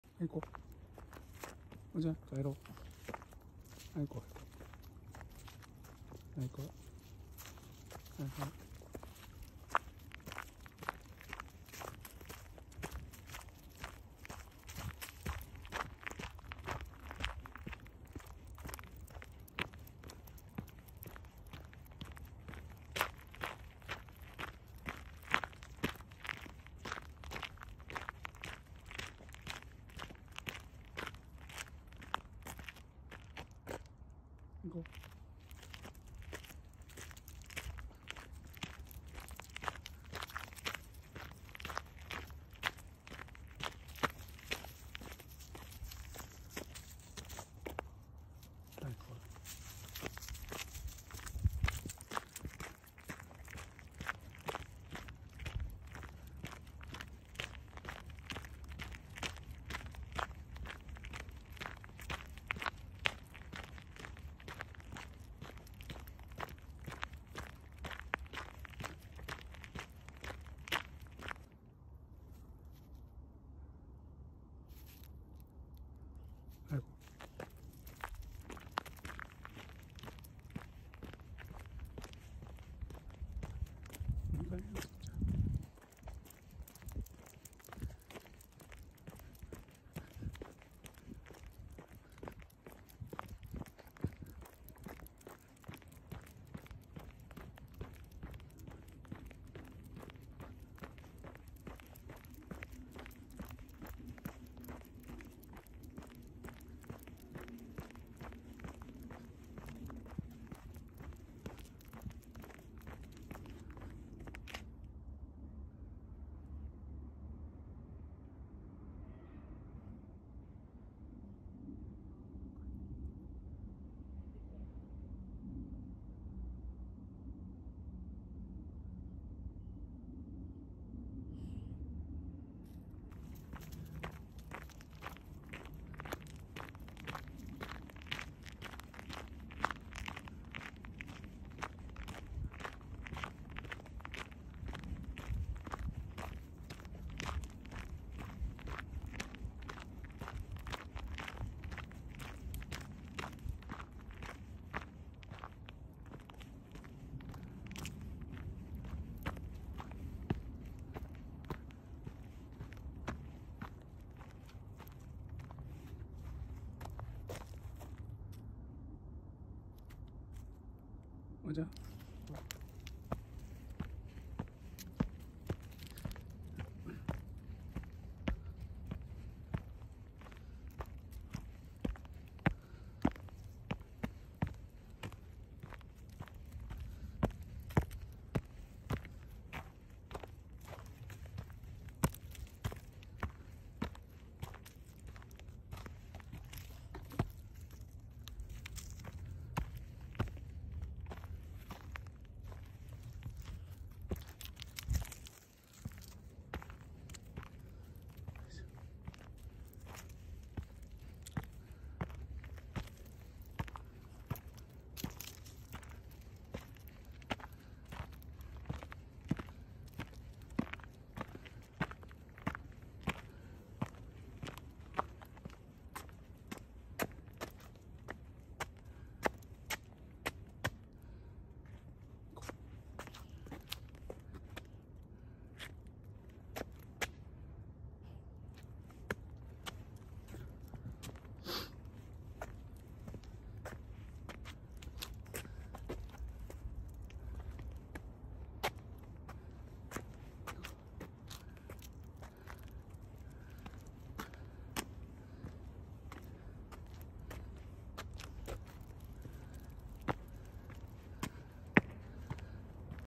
【豆柴ここちゃん】夜のお散歩
今年に入って、夜にお散歩中に携帯電話に連絡がありブラブラ歩いていたら、ここちゃんのリードを離していて、真っ暗の中でここちゃんを探すというハプニングがありました。それで、夜のお散歩にはライトを付けていくことになりました。

Earlier this year, when I was taking a walk at night, I got a call on my cell phone, and when I was walking around, I let go of coco's leash, and there was an incident where I searched for coco in the dark.  That's why I've decided to turn on the lights for my walks at night.